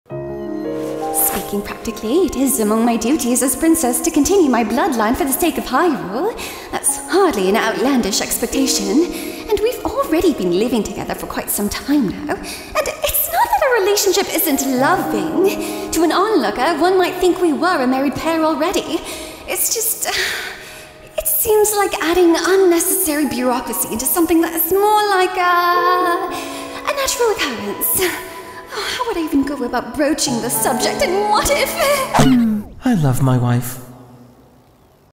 Married - Tears of the Kingdom
Zelda over here trying to figure out the proposal for something already done.

AderuMoro (Heidi Tabing) as Princess Zelda

Me... as Link